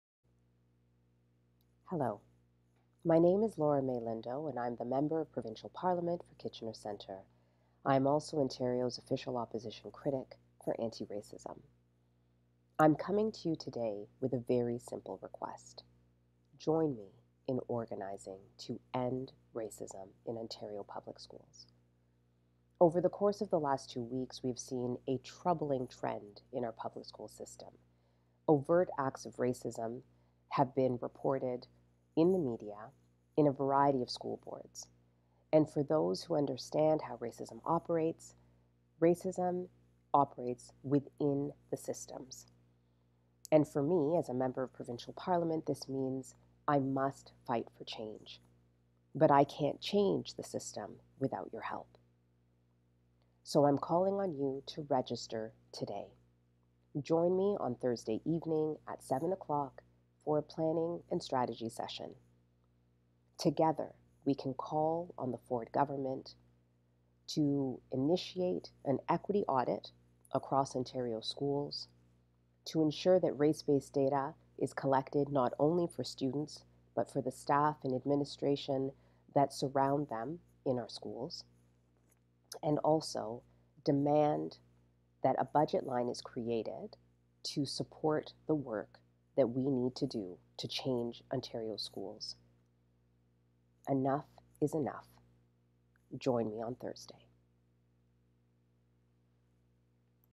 End Racism In Ontario Schools NOW!
It's time to organize. Let's call on the government to invest in ending racism in Ontario schools. Join me on Thursday November 4th at 7pm for a strategy session.